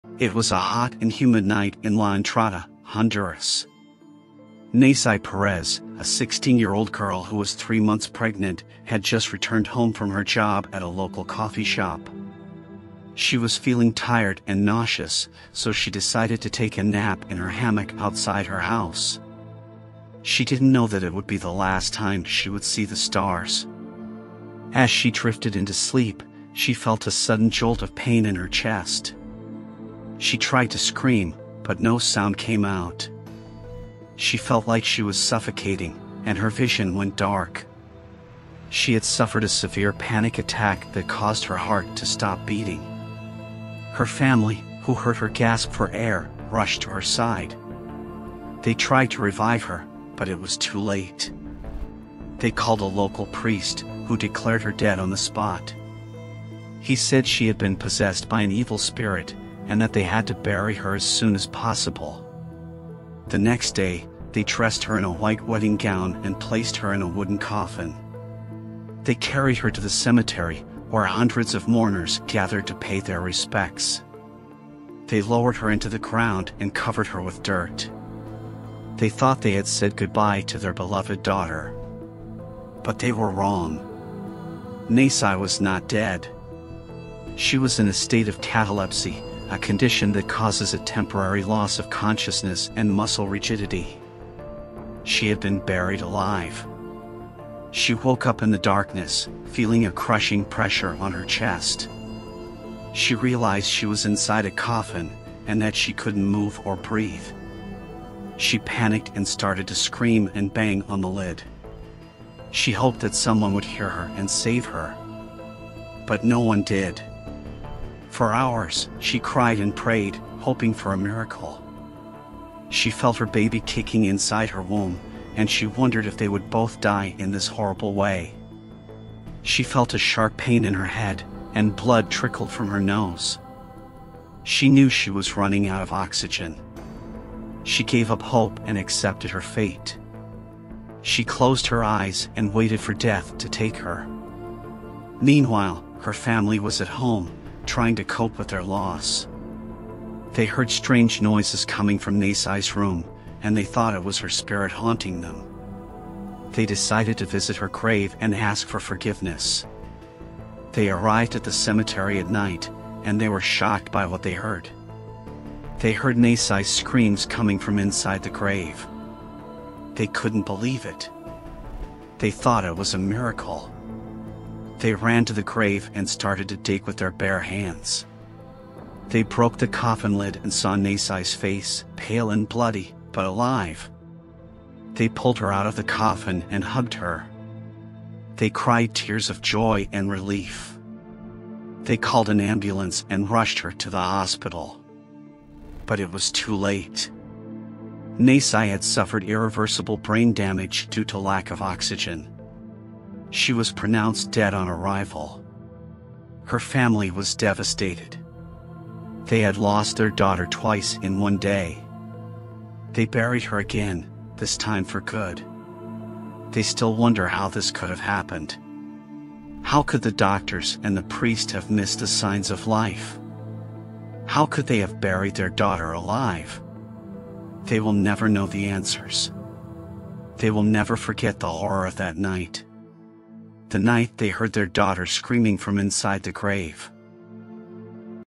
The Horrifying Story of the Girl Who Was Buried Alive and Woke Up in Her Grave!
The Horrifying Story of the Girl Who Was Buried Alive and Woke Up in Her Grave! | ASMR | Horror Story | True Story | Serial killer | Murder | Tragedy

It was a hot and humid night in La Entrada, Honduras. Neysi Perez, a 16-year-old girl who was three months pregnant, had just returned home from her job at a local coffee shop. She was feeling tired and nauseous, so she decided to take a nap in her hammock outside her house.
She didn’t know that it would be the last time she would see the stars.
As she drifted into sleep, she felt a sudden jolt of pain in her chest. She tried to scream, but no sound came out. She felt like she was suffocating, and her vision went dark.